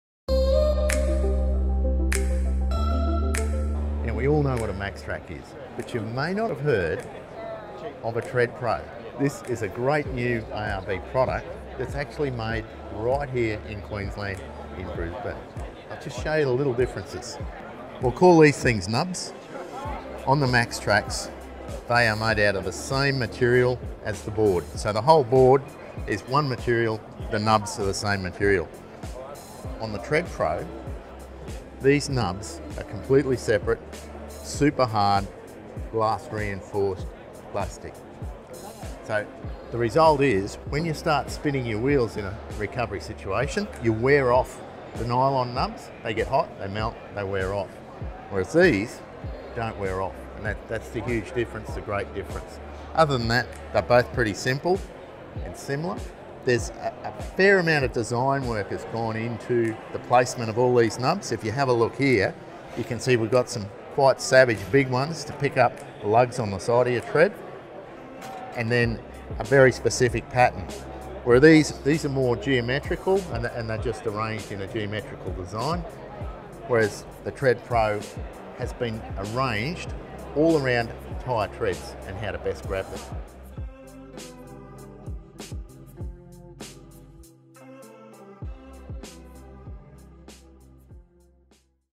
ARB Tred Pro Recovery Kit
Roger Vickery showcases the Tred Pro from ARB 4×4 Accessories. Designed especially for when you find yourself stuck off road without any help in sight. For Info about the ARB Tred Pro